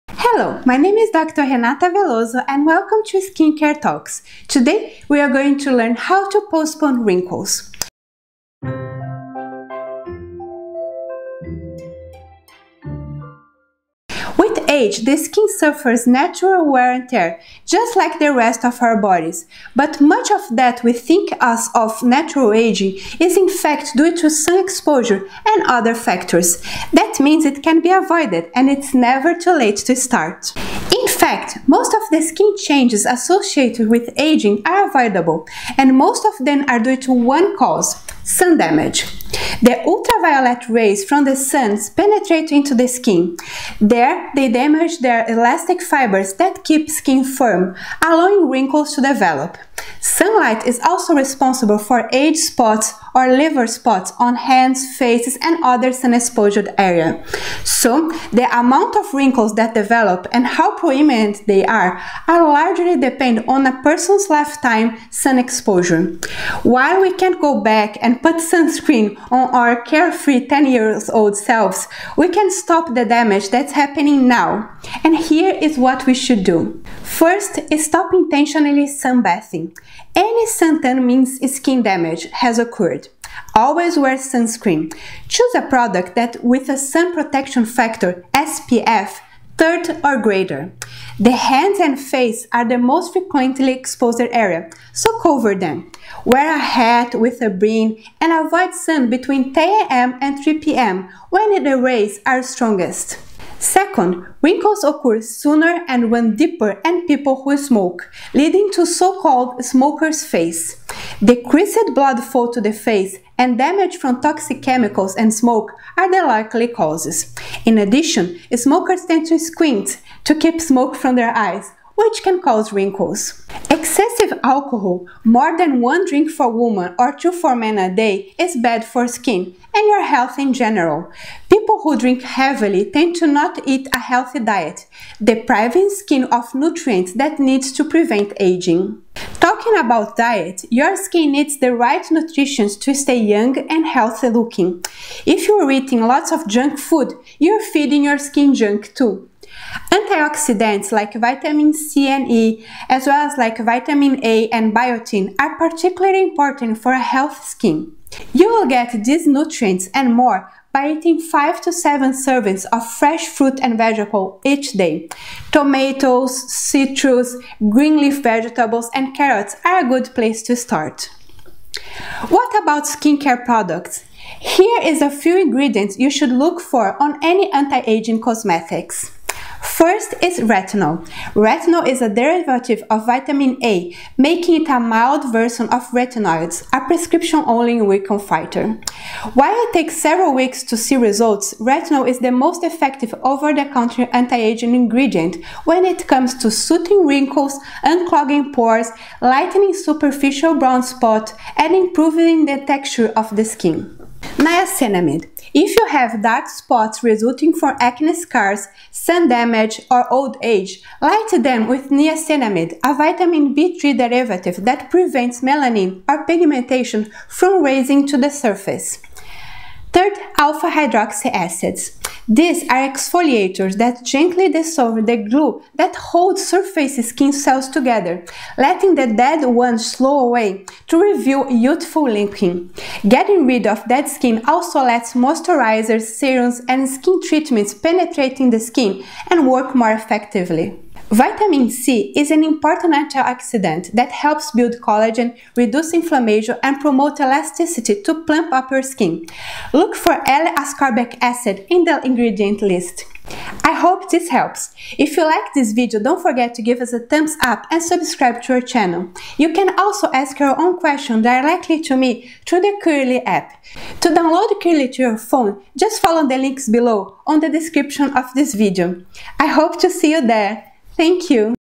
Anti-aging tips and products
To ask you personal question download Curely App

With age, the skin suffers natural wear-and-tear, just like the rest of our bodies. But much of what we think of as natural aging is in fact due to sun exposure and other factors. That means it can be avoided -- and it's never too late to start.

In fact, most of the skin changes associated with aging are avoidable. And most of them are due to one cause: sun damage.

The ultraviolet rays from the sun penetrate into the skin. There, they damage the elastic fibers that keep skin firm, allowing wrinkles to develop. Sunlight is also responsible for age spots or "liver spots" on the hands, face, and other sun-exposed areas. So, the amount of wrinkles that develop, and how prominent they are, are largely dependent on a person's lifetime sun exposure. While we can't go back and put sunscreen on our carefree 10-year-old selves, we can stop the damage that's happening now. Here is what you should do:

Stop intentionally sunbathing. Any suntan means skin damage has occurred. Always wear sunscreen. Choose a product with sun protection factor (SPF) 30 or greater. The hands and face are the most frequently exposed -- cover them. Wear a hat with a brim and avoid the sun between 10 a.m. and 3 p.m., when its rays are the strongest.

Second wrinkles occur sooner and run deeper in people who smoke, leading to so-called "smoker's face." Decreased blood flow to the face, and damage from toxic chemicals in smoke, are the likely causes. In addition, smokers tend to squint to keep smoke from their eyes, which can cause wrinkles.

Excessive alcohol (more than one drink a day for women, two for men) is bad for skin, and your health in general. People who drink heavily tend to not eat a healthy diet -- depriving skin of the nutrients it needs to prevent aging.

Talking about diet, Your skin needs the right nutrition to stay young and healthy looking. If you're eating lots of junk food, you're feeding your skin junk, too. Antioxidants like vitamin C and E, as well as vitamin A and the B vitamin biotin, are particularly important for healthy skin. You'll get all of these nutrients, and more, by eating five to seven servings of fresh fruits and vegetables each day. Tomatoes, citrus, green leafy vegetables, and carrots are a good place to start.

What about skincare products? Here is a few ingredients you should look for on anti-aging cosmetics:

Retinol
Retinol is a derivative of vitamin A, making it a milder version of retinoids (a prescription-only wrinkle fighter). While it takes several weeks to see results, retinol is the most effective over-the-counter anti-aging ingredient when it comes to "smoothing wrinkles, unclogging pores, lightening superficial brown spots, and improving the texture of the skin.

Niacinamide
If you have dark spots resulting from acne scars, sun damage, or old age, lighten them with niacinamide, a vitamin-B3 derivative that prevents melanin, or pigmentation, from rising to the surface. "It may help to improve the skin's moisture barrier and collagen production, all of which reverses the appearance of sun damage. Plus, it's known to reduce inflammation, and even acne.

Alpha Hydroxy Acids
Alpha hydroxy acids are exfoliators that gently dissolve the "glue" that holds surface skin cells together, letting the dead ones slough away to reveal youthful looking skin. This process encourages cell turnover, which typically slows with age. Getting rid of dead skin also lets moisturizers, serums, and skin treatments penetrate the skin and work more effectively.

Vitamin C is important antioxidant that helps build collagen, reduce inflammation, and promote elasticity to plump up skin. But how can you tell if vitamin C in a product is stable? Look for "L-ascorbic acid" in the ingredient list.